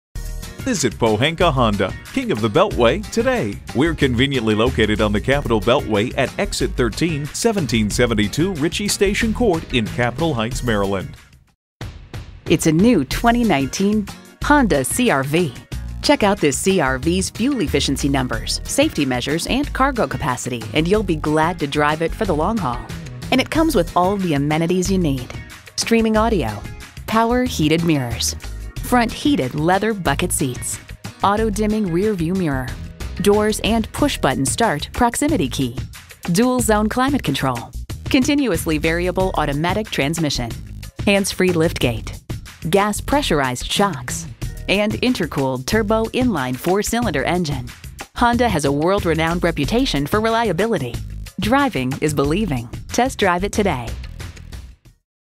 New 2019 Honda CR-V Washington DC Honda Dealer, MD HKH637638 - SOLD
Year: 2019
Make: Honda
Model: CR-V
Trim: Touring
Engine: 1.5L I4 DOHC 16V
Transmission: CVT
Color: Beige
Interior: ivory
Stock #: HKH637638
VIN: 2HKRW2H97KH637638
sandstorm metallic 2019 Honda CR-V Touring AWD CVT 1.5L I4 DOHC 16V AWD.brbr27/33 City/Highway MPG

Address:
1772 Ritchie Station Court Capitol Heights